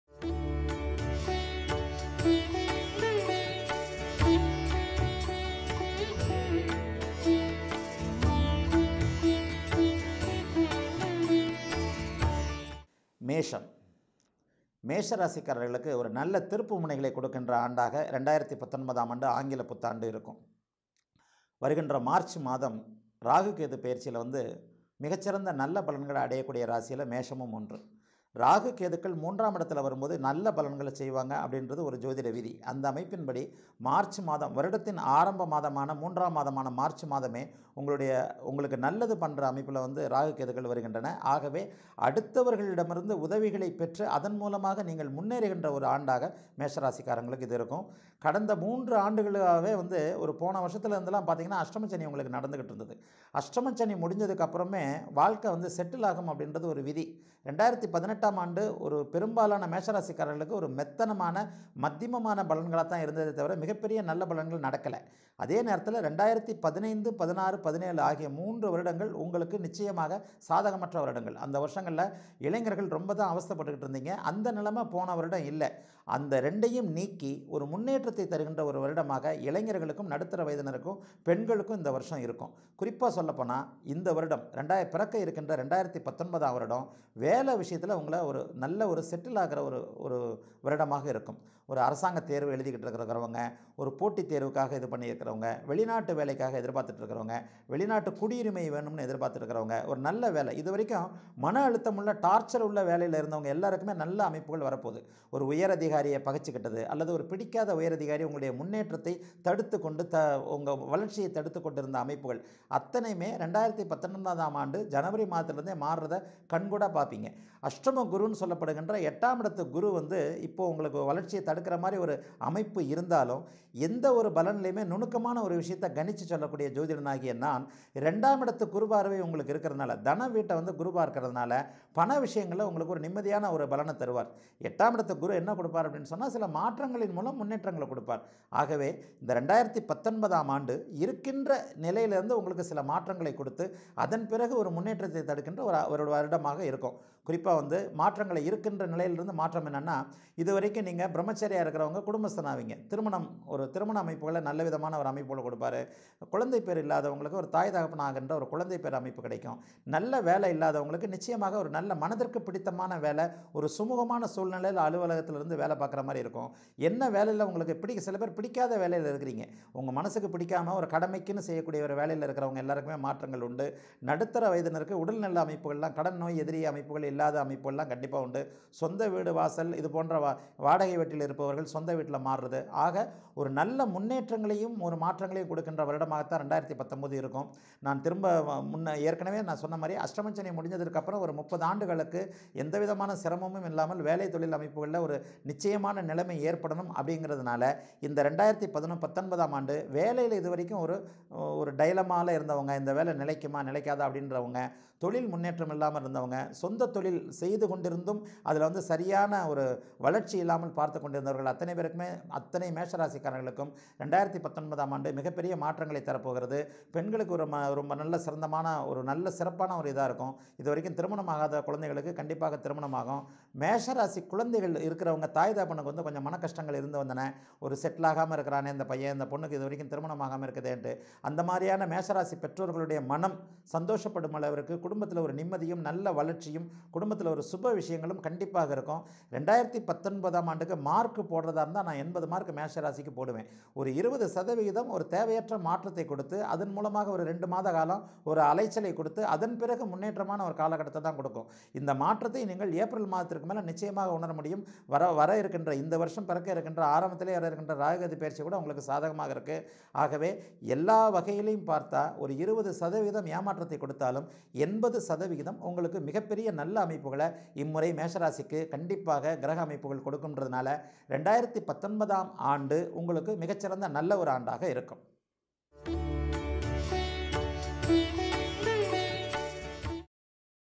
0369 - Mesham: 2019 New Year Palankal - மேஷம்: 2019 ஆங்கில புத்தாண்டு பலன்கள்adityaguruji
2019 ம் ஆண்டு மேஷ ராசிக்கு என்ன பலன்கள் நடக்கும்? குருஜியின் புகழ்பெற்ற ஆண்டு பலன்கள்..- குருஜியின் You Tube வீடியோ.